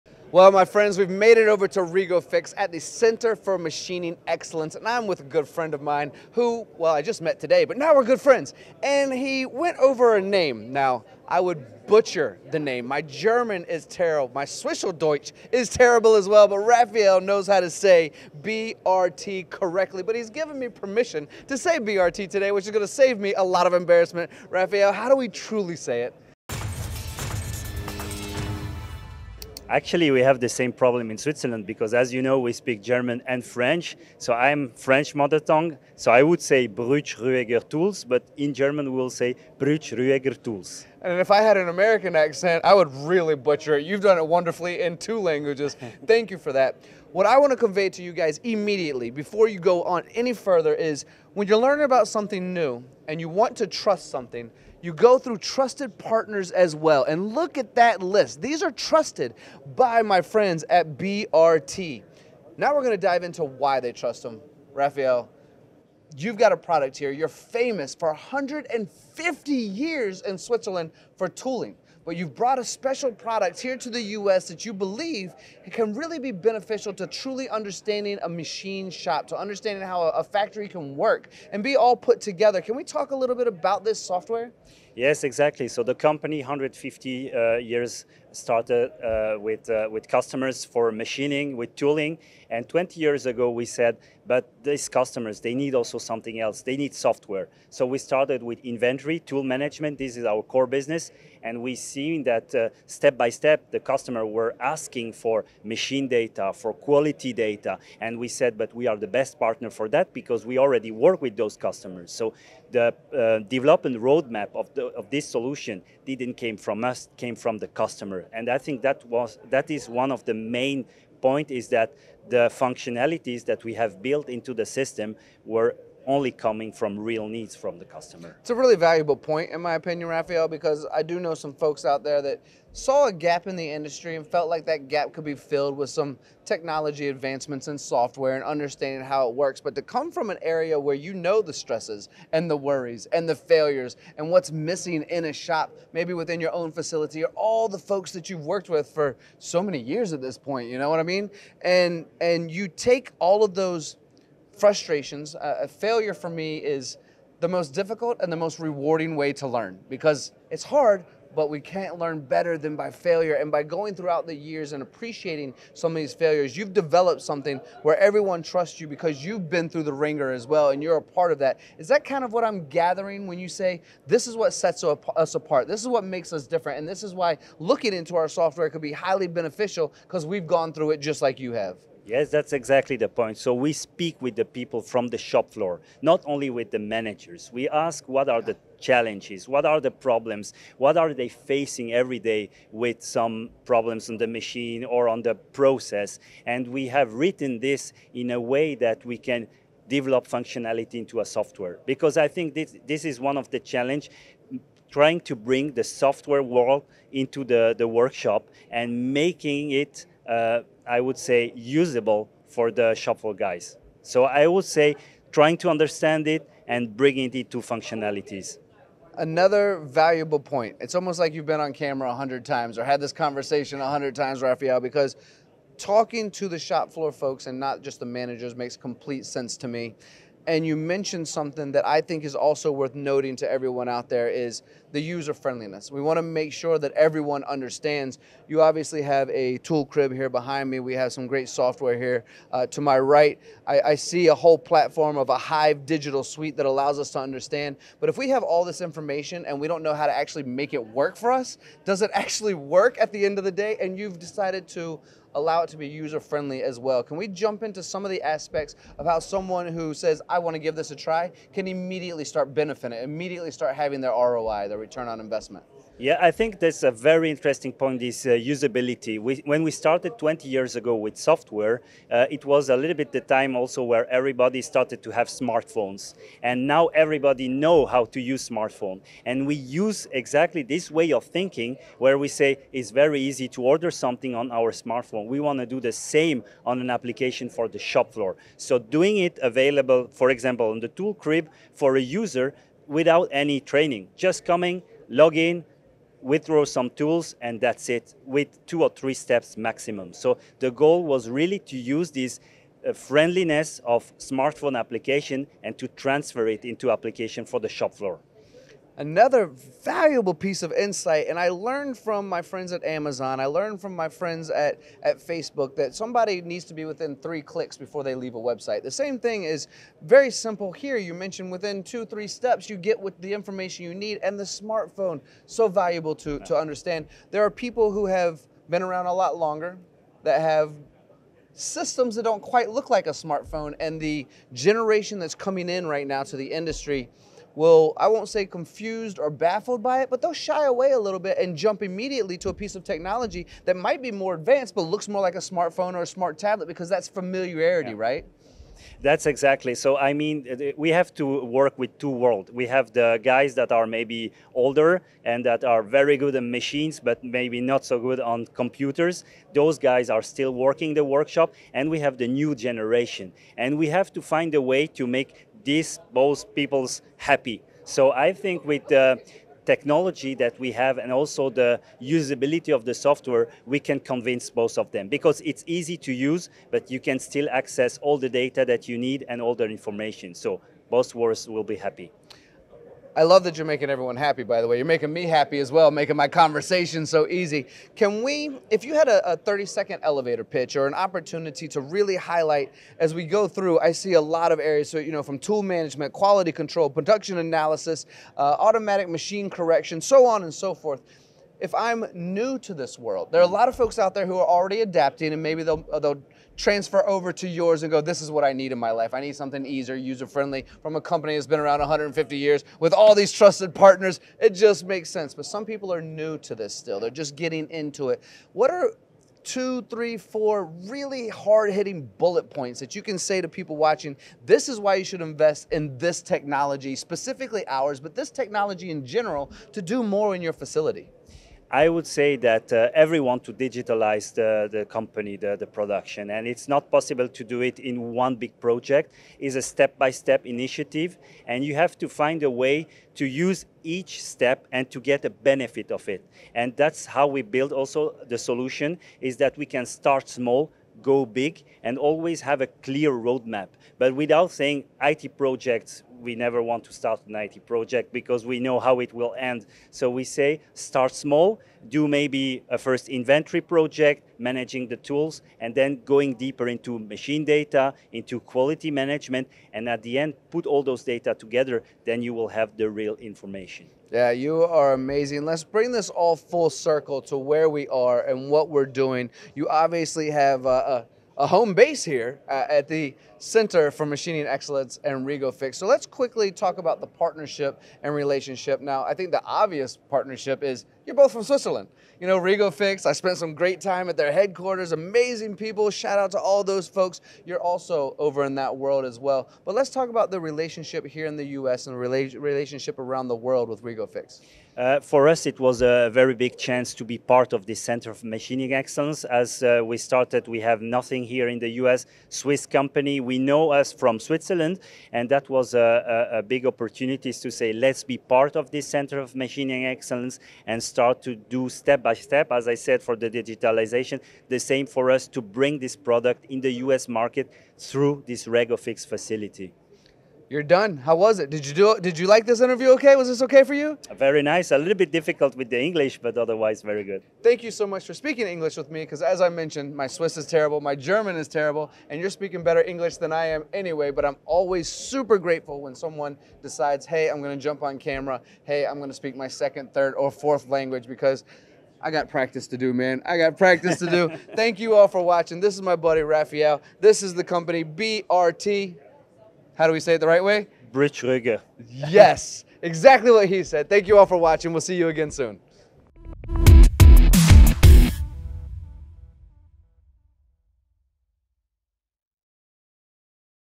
Tradition Meets Technology – 150 Years Behind the Digital Future
“The development roadmap of this solution didn't come from us it came from the customer.”

At the Rego-Fix Center for Machining Excellence, Tony Gunn of MTDCNC speaks with Raphaël Müller of Brütsch Rüegger Tools (BRT) about a revolutionary shopfloor software built with machinists, for machinists.

Designed with the simplicity of a smartphone and the scalability of true Industry 4.0, this solution bridges the gap between traditional shopfloor knowledge and cutting-edge digital tools all without the complexity of large IT deployments.

This is digitalisation done right: practical, intuitive, and focused on ROI. See how BRT is helping manufacturers around the world unlock value from the data they already generate.

Ready for the next title when you are.

Ask ChatGPT